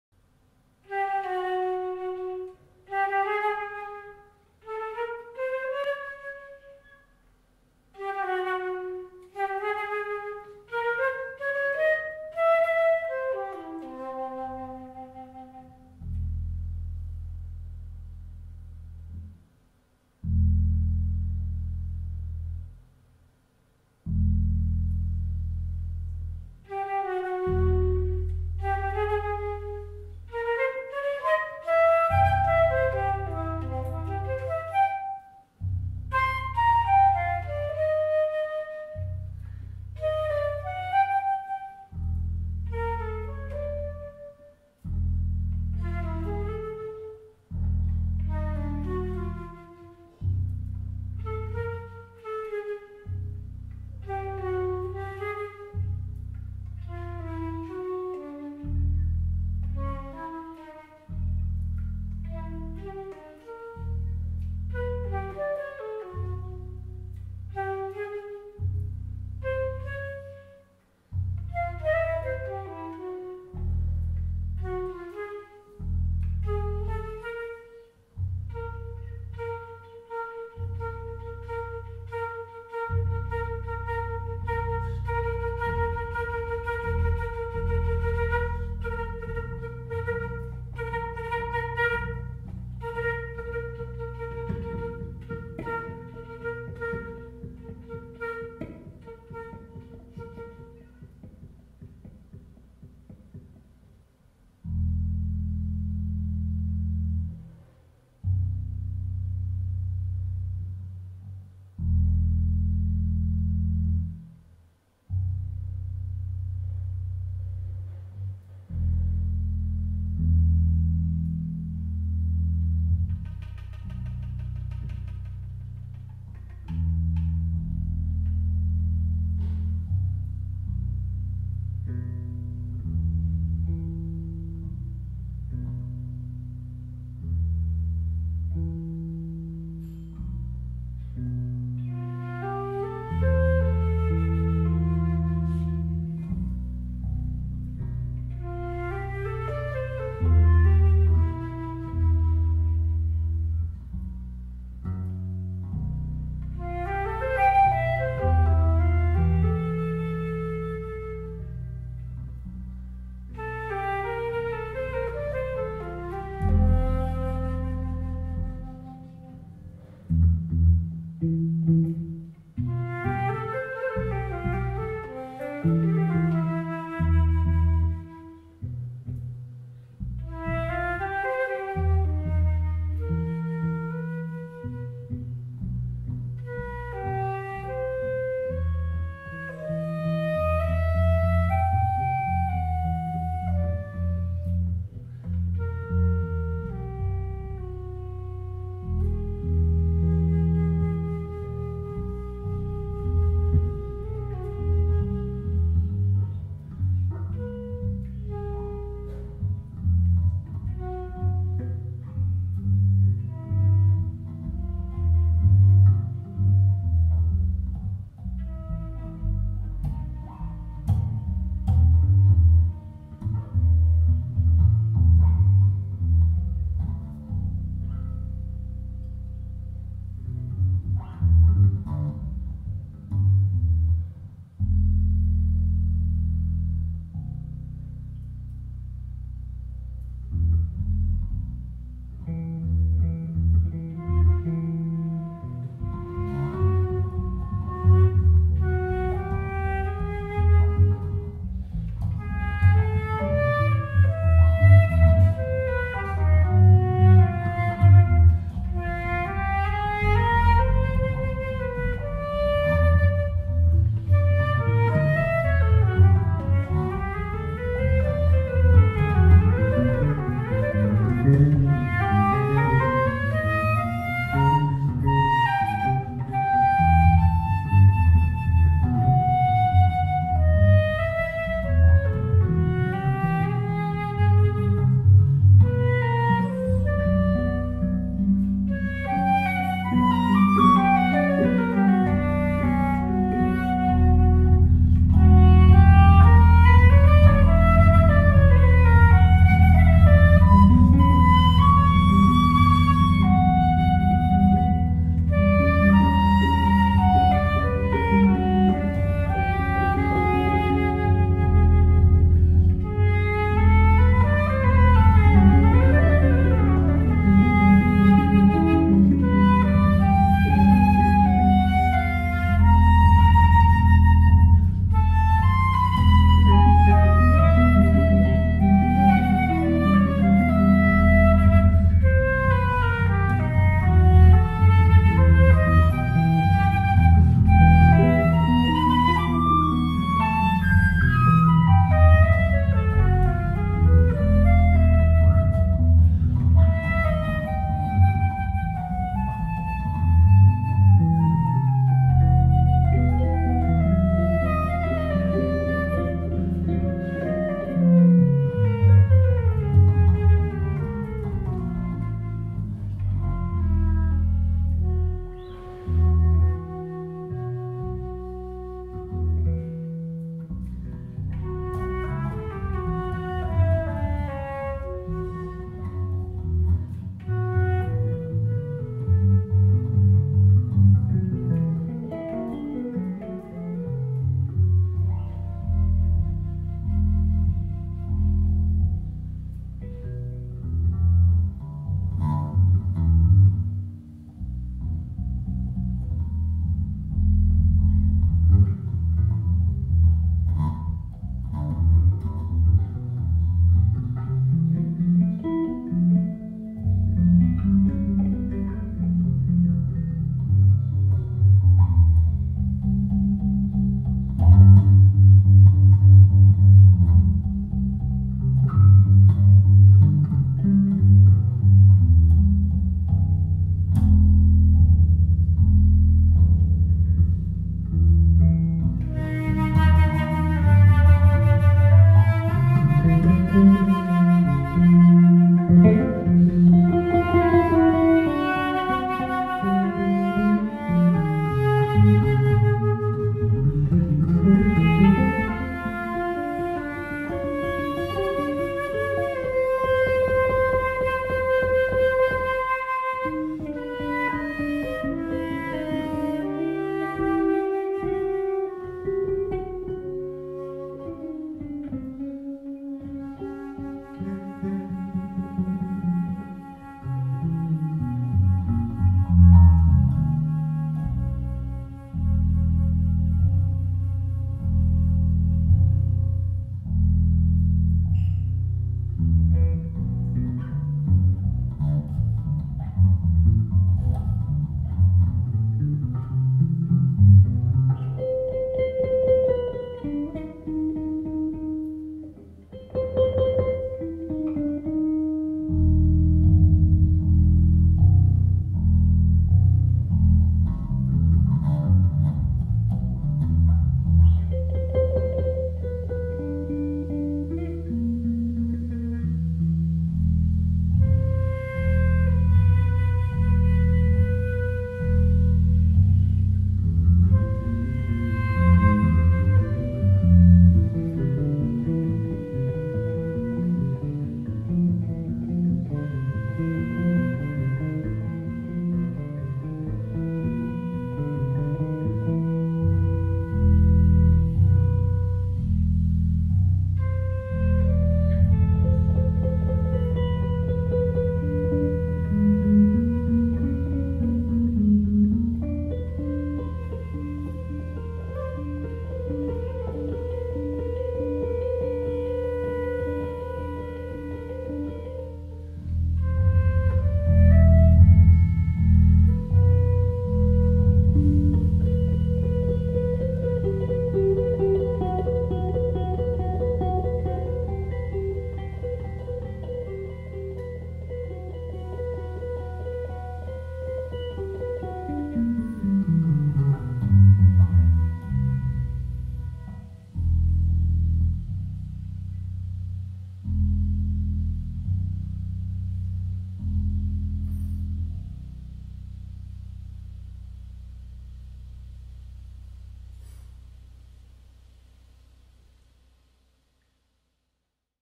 Music In Motion 109 - 20.10.2014
Francesco Pirolo - electric bass & effects

free improvisation